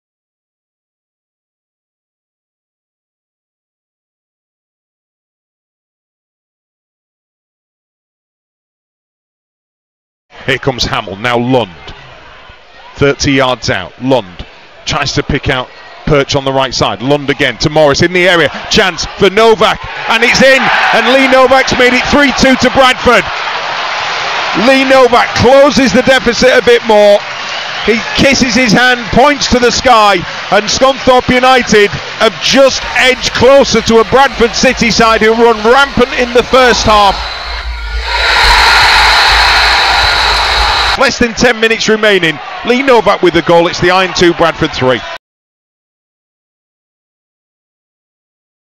⚽ Goal: Lee Novak v Bradford City
Another look at Lee Novak's 13th goal of the season against Bradford City.

📺 Watch interviews and much more on @iFollowIron. Sign-up details/what's on offer